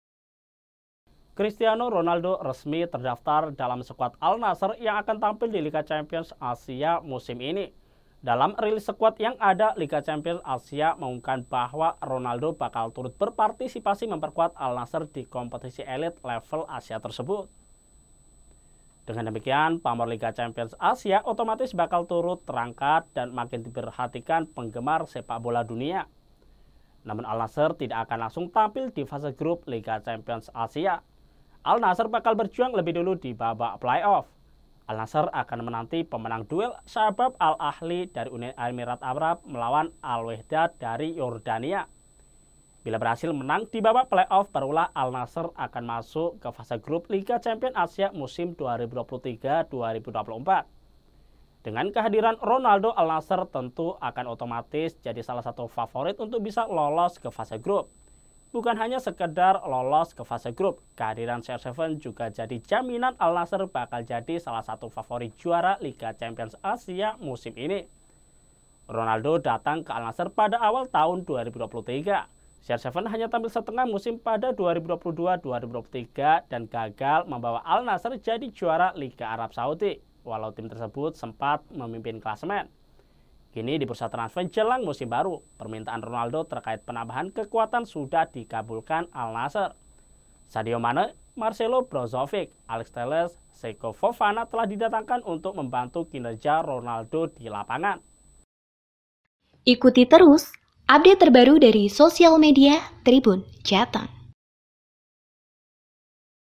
Wow, Cristiano Ronaldo Bela Al Nassr di Liga Champions Asia
Cristiano Ronaldo resmi terdaftar dalam skuad Al Nassr yang tampil di Liga Champions Asia musim ini.

Dalam rilis skuad yang ada, Liga Champions Asia mengumumkan bahwa Ronaldo bakal turut berpartisipasi memperkuat Al Nassr di kompetisi elite level Asia tersebut.

Dengan demikian, pamor Liga Champions Asia otomatis bakal turut terangkat dan makin diperhatikan penggemar sepak bola dunia.

Namun Al Nassr tidak akan langsung tampil di fase grup Liga Champions Asia.

Al Nassr bakal berjuang lebih dulu di babak playoff.

Al Nassr akan menanti pemenang duel Shabab Al Ahli (Uni Emirat Arab) melawan Al Wehdat (Yordania).

Bila berhasil menang di babak playoff, barulah Al Nassr akan masuk fase grup Liga Champions Asia musim 2023/2024.

Dengan kehadiran Ronaldo, Al Nassr tentu akan otomatis jadi salah satu favorit untuk bisa lolos ke fase grup.

Bukan hanya sekadar lolos fase grup, kehadiran CR7 juga jadi jaminan Al Nassr bakal jadi salah satu favorit juara Liga Champions Asia musim ini.

Ronaldo datang ke Al Nassr pada awal tahun 2023.

CR7 hanya tampil setengah musim pada 2022/2023 dan gagal membawa Al Nassr jadi juara Liga Arab Saudi walaupun tim tersebut sempat memimpin klasemen.

Kini di bursa transfer jelang musim baru, permintaan Ronaldo terkait penambahan kekuatan sudah dikabulkan Al Nassr.

Sadio Mane, Marcelo Brozovic, Alex Telles, dan Seko Fofana telah didatangkan untuk membantu kinerja Ronaldo di lapangan.